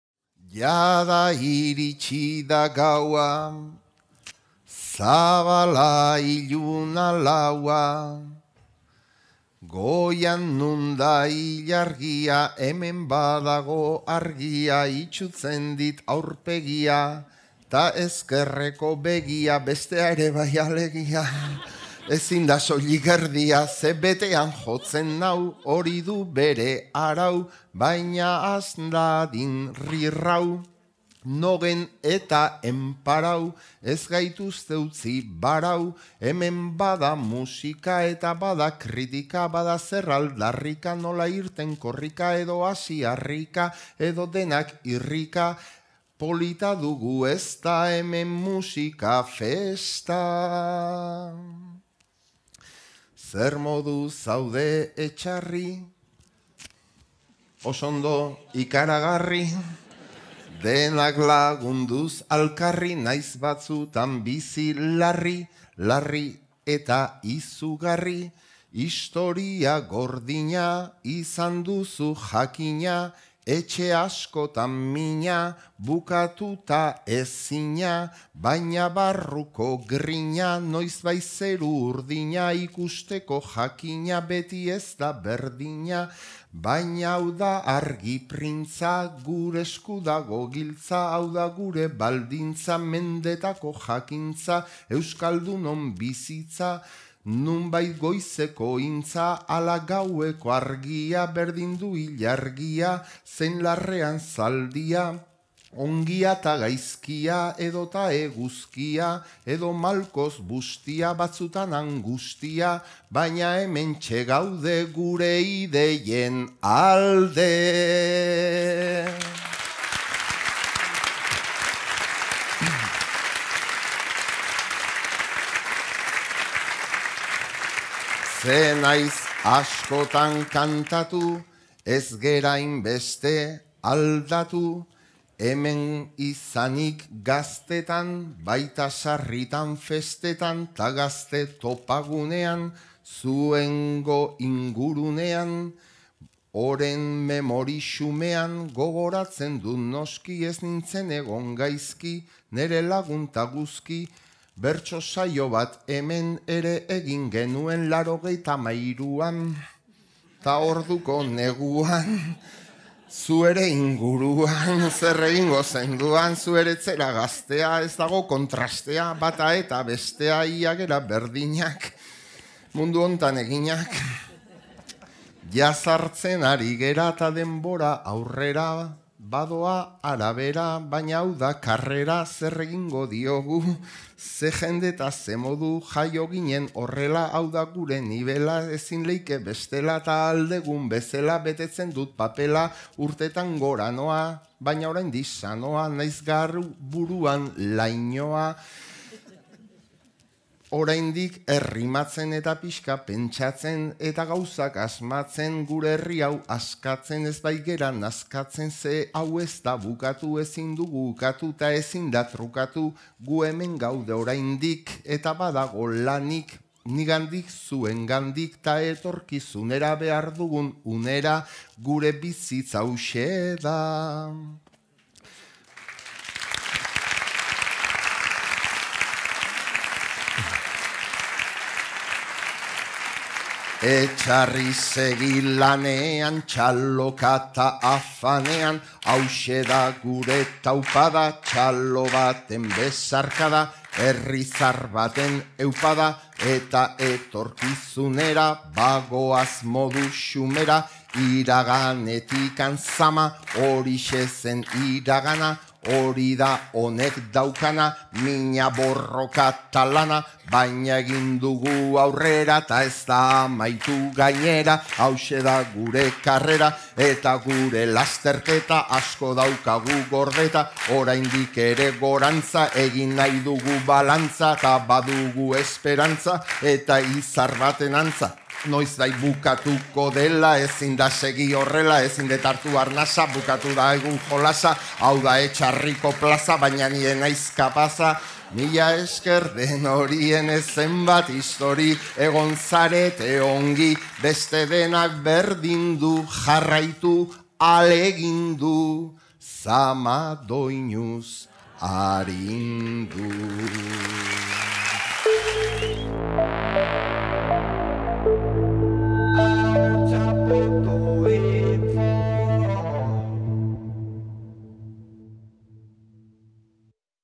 JON MAIA ZAMA DOINUZ ARINDU VIII
Etxarri Aranatzeko Zama Doinuz Arindu elkartasun jaialdiaren VIII. edizioa, euskal preso, iheslari eta deportatuen eskubideen alde. Kalakan, Lorelei, Nogen, Olaia Inziarte, Kerobia, Iñigo (ETS), Tatxers eta Ion Maia izan ziren.